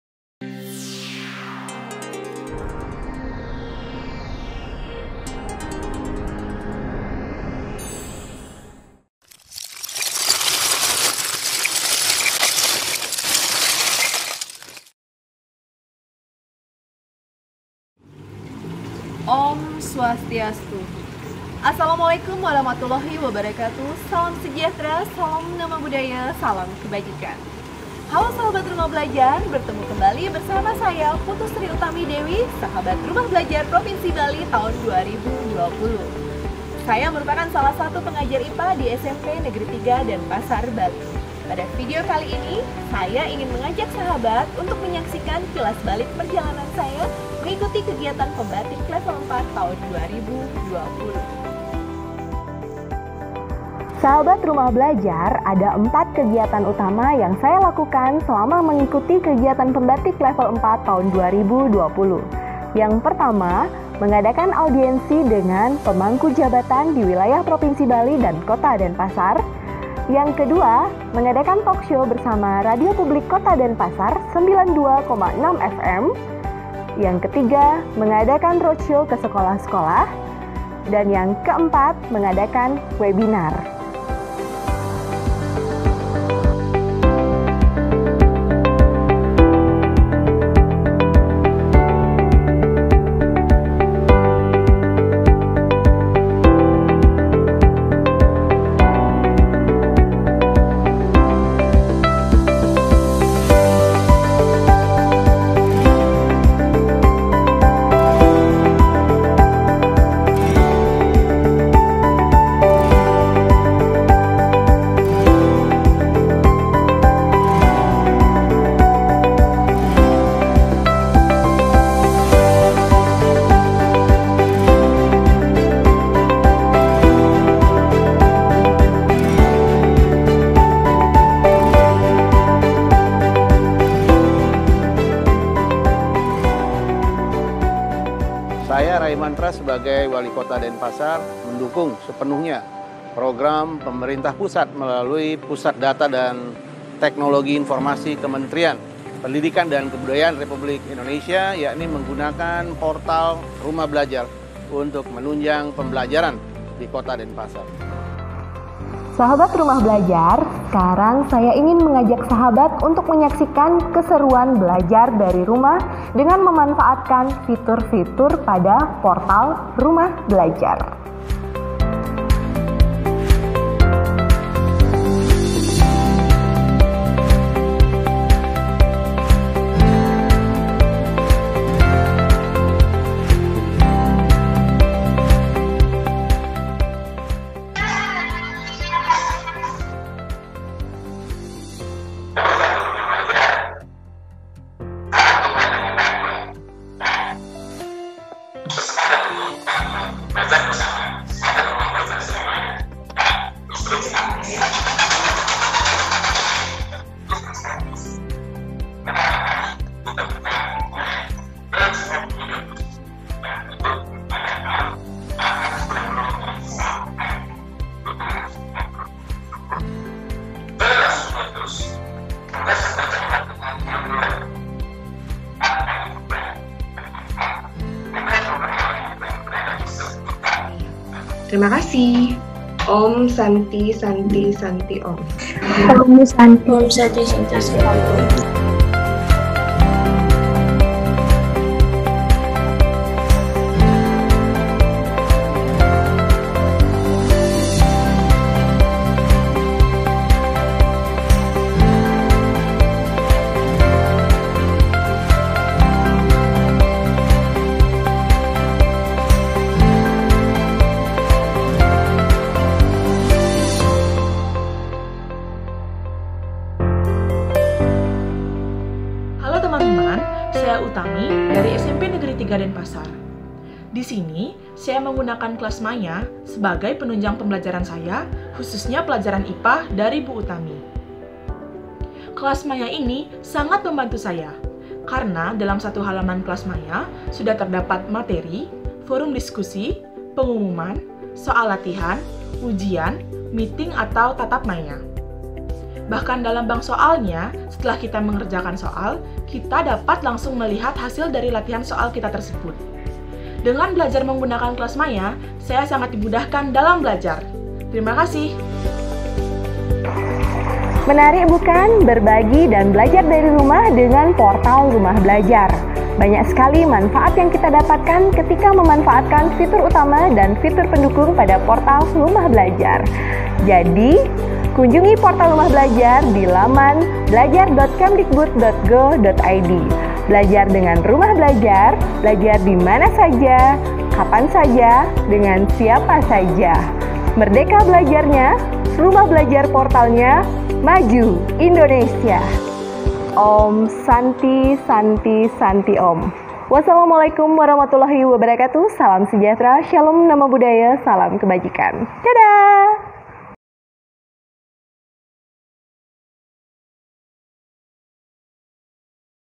SRB BALI 2020
Suatu keberuntungan bisa diberikan kesempatan untuk menjadi bagian dari pembaTIK level 4 tahun 2020 di provinsi Bali. Pengalaman luar biasa yang saya dapatkan dari rentang waktu 14 September 2020 sampai dengan 21 Oktober 2020.

Kegiatan pembaTIK level 4 diawali dengan kuliah umum lanjut dengan coaching, dan kegiatan berbagi. Ada 4 program utama saya pada pembaTIK level 4 ini, di antaranya Audiensi, Talkshow, Roadshow, dan Webinar.

Pada kesempatan ini ijinkan saya mengucapkan terima kasih yang sebesar-besarnya pada Pusdatin Kemdikbud RI yang sudah menyediakan wadah bagi guru-guru dalam mengembangkan kemampuan TIK dan berbagi ❤️

Saya juga ingin menyampaikan terima kasih  kepada Bapak Walikota Denpasar beserta jajaran di lingkungan pemerintah kota Denpasar dan juga jajaran di dinas dikpora provinsi Bali yang sudah membimbing, memotivasi, mendukung, dan mendoakan kami untuk sukses menjalani tugas di PembaTIK Level 4 Tahun 2020 🙏

Terima kasih juga kepada Coach Bapak Sumarno, Mas Hafiz Maulana, Mas Ari Nugraha, DRB N Bali 2017, 2018, 2019, dan juga DRB versi jejak Bali. Kepada rekan-rekan SRB Bali 2020 yang selalu bersedia merangkul saya. Semoga kekeluargaan ini bisa berlanjut ❤️

Tak lupa juga saya ucapkan kepada rekan-rekan guru dan pegawai di SMP Negeri 3 Denpasar, di SMP PGRI 6 Denpasar, SMP Pelangi Dharma Nusantara, SD N 18 Pemecutan, dan SD N 29 Dangin Puri atas kesempatan yang diberikan untuk mengadakan sosialisasi tatap maya 🙏

Terima kasih juga untuk murid-muridku 9A, 9B, 9C, 7J TA 2020/2021 serta kakak-kakak alumni spentri🎉

Terakhir ucapan terima kasih untuk keluarga tercinta ❤️ yang selalu mendukung setiap langkah dalam pengembangan karir mama 🙏

Semangat berbagi❤️
Belajar dengan rumah belajar
Belajar di mana saja
Kapan saja
Dengan siapa saja

Merdeka Belajarnya
Rumah Belajar Portalnya
Maju Indonesia